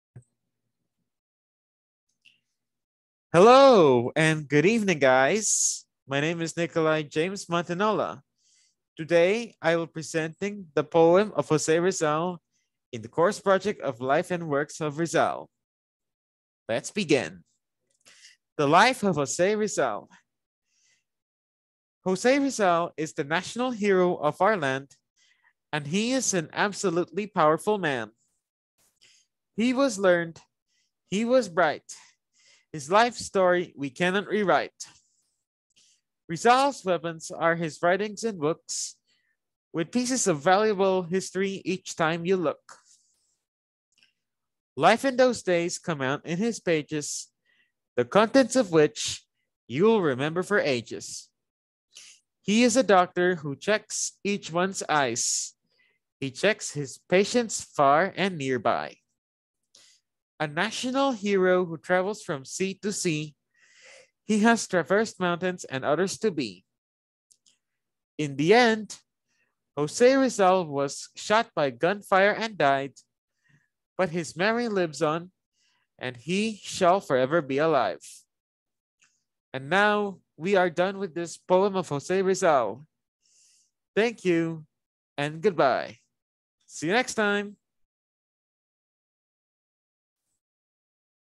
POEM OF JOSE RIZAL (FOR THE COURSE PROJECT OF LIFE AND WORKS OF RIZAL)
This poem of Jose Rizal was recorded on Zoom last night. After this, I will prepare to sing Jose Rizal's song before taking the final exam!

Thank you and I hope you enjoy it, guys!